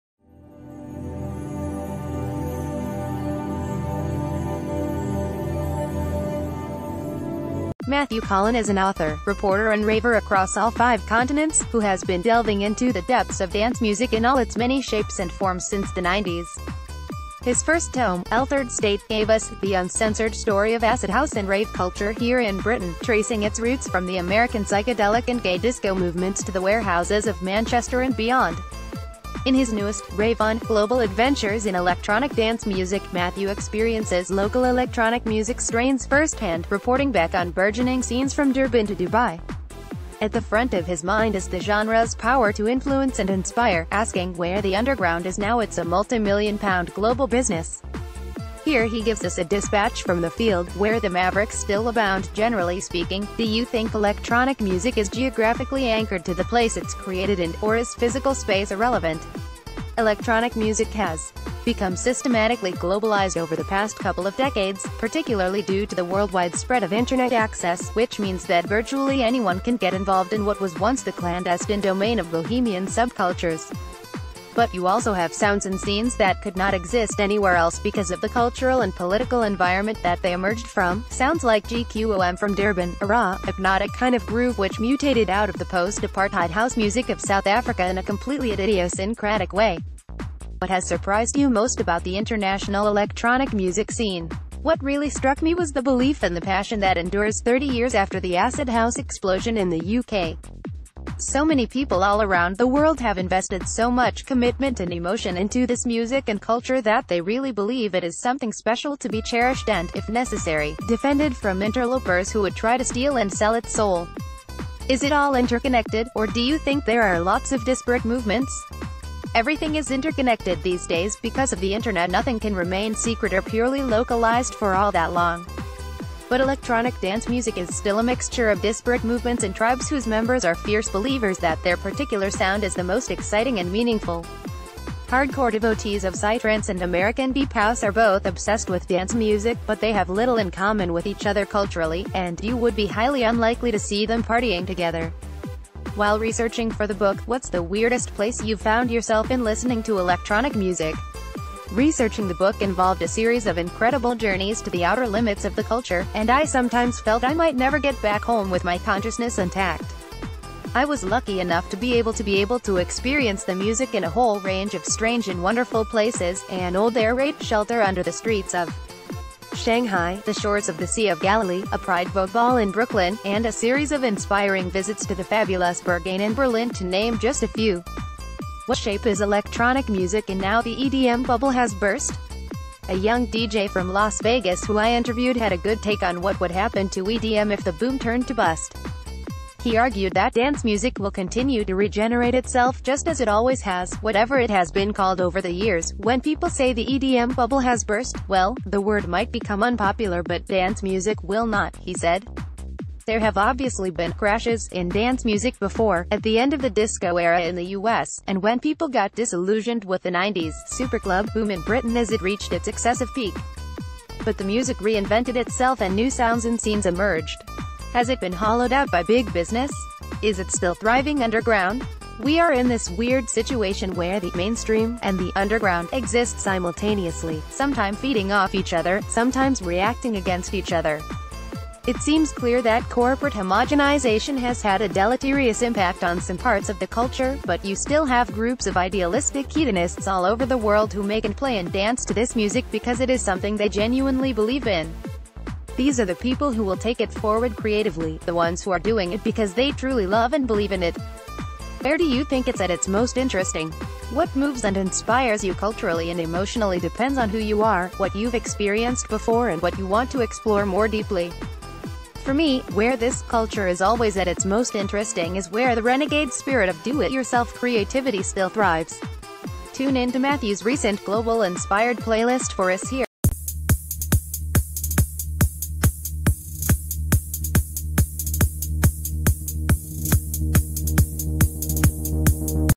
Breaking News | Interview: Matthew Collin
Matthew Collin is an author, reporter and raver across all five continents, who has been delving into the depths of dance music in all its many shapes and forms since the nineties. His first tome, Altered State, gave us the uncensored story of acid house and rave culture here in Britain, tracing its roots from the American psychedelic and gay disco movements to the warehouses of Manchester and beyond. In his newest, Rave On: Global Adventures in Electronic Dance Music, Matthew experiences local ...